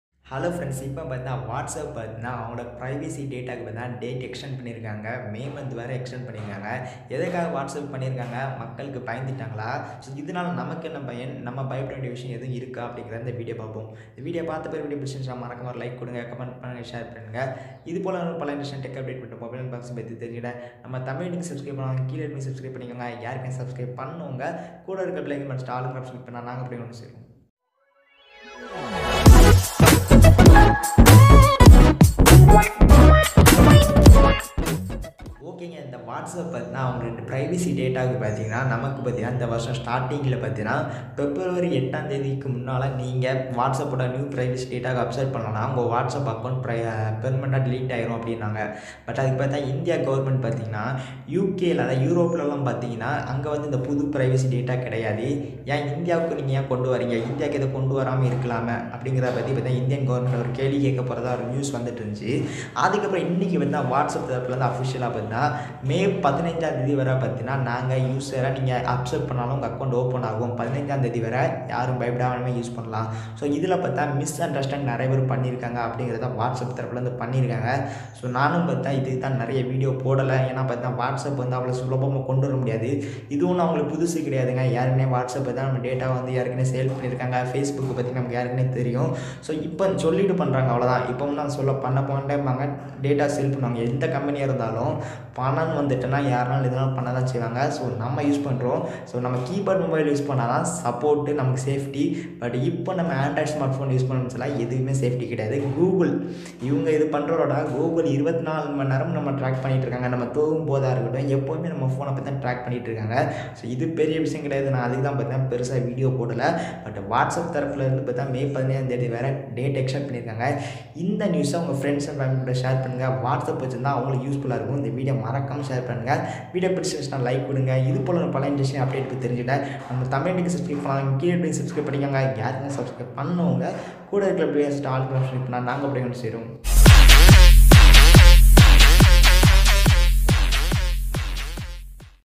பிப்ரவரி 8ஆம் தேதி பிறகும் WhatsApp வேலை செய்யும்
WhatsApp வருகிற மே மாதம் 15ஆம் தேதி வரை வேலை செய்யும் என்று அந்நிறுவனம் அறிவித்துள்ளது.